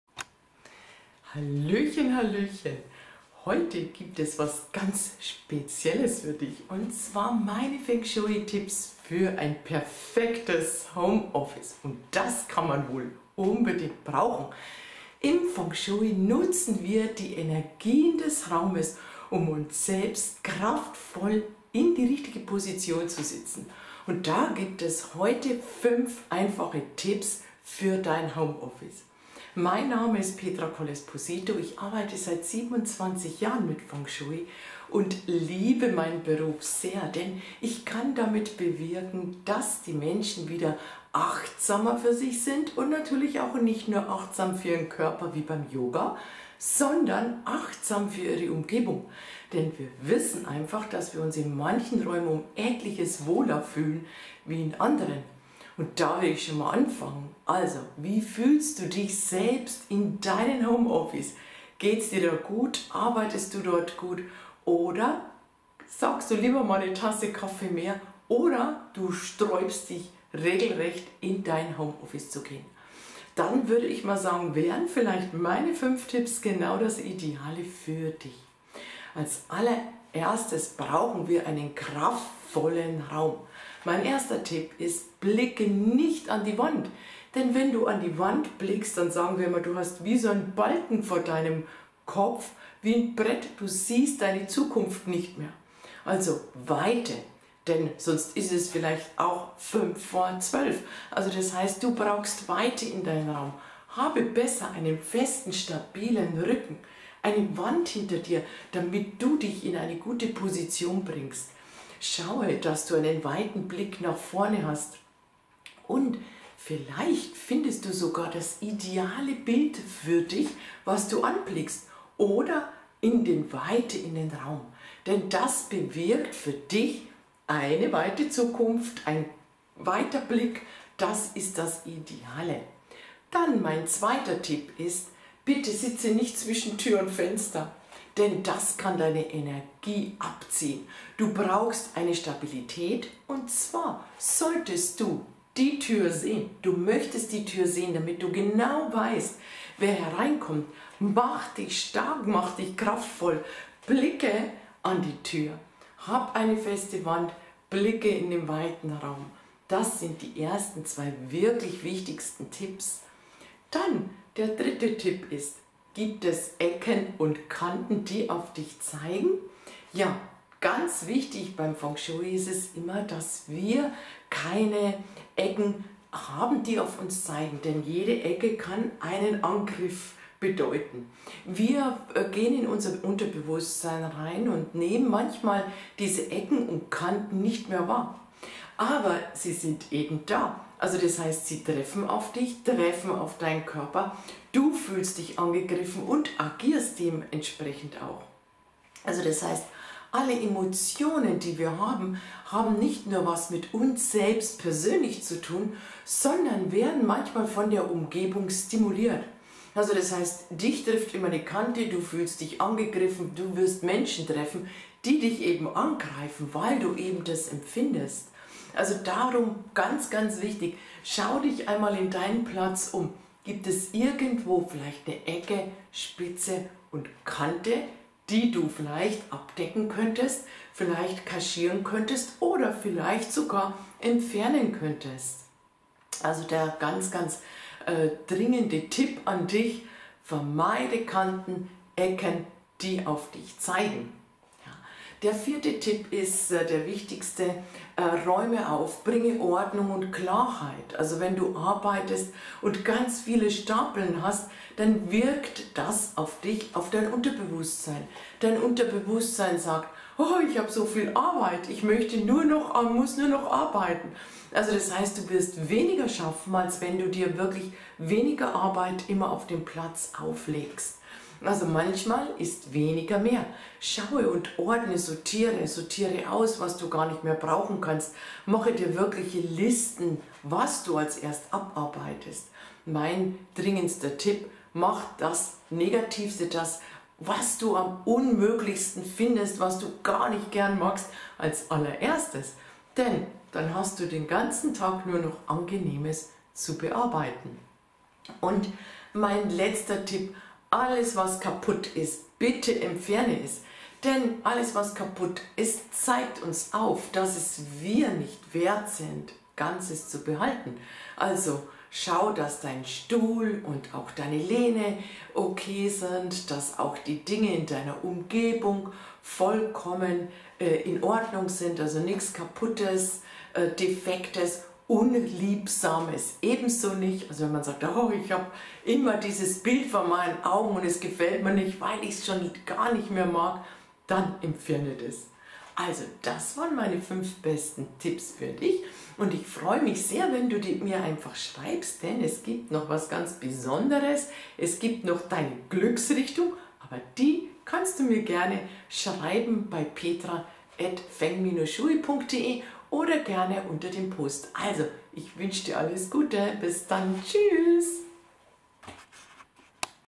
Produktiver Arbeitsplatz
Wie du produktiver arbeitest und dich dabei entspannen kannst, erfährst du in diesem Video.